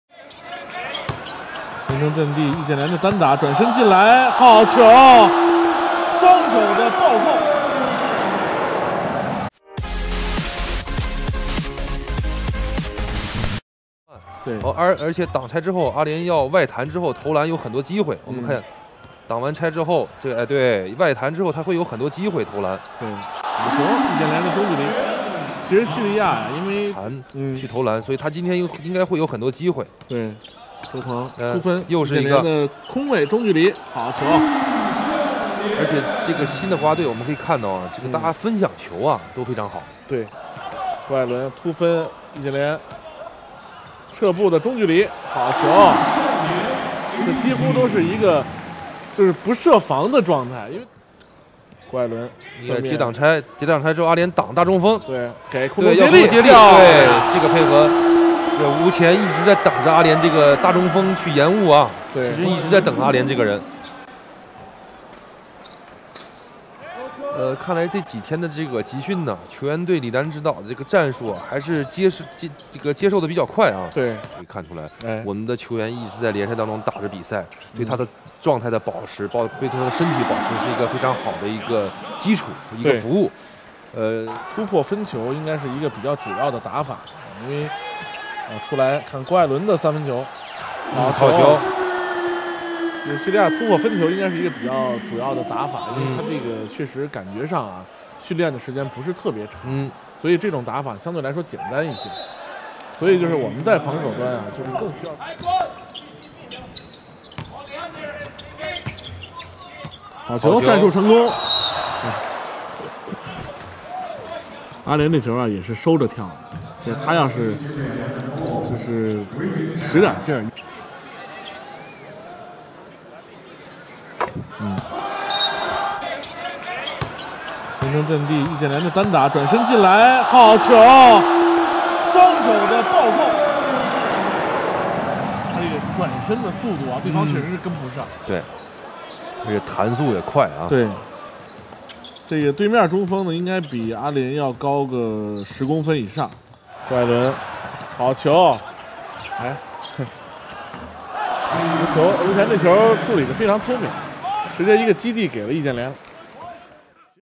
易建联 (Yi Jianlian) Full Highlights vs Syria Fiba World Cup Qualifiers | 易建联世预赛集锦
易建联回来啦！
对决叙利亚集锦15分7篮板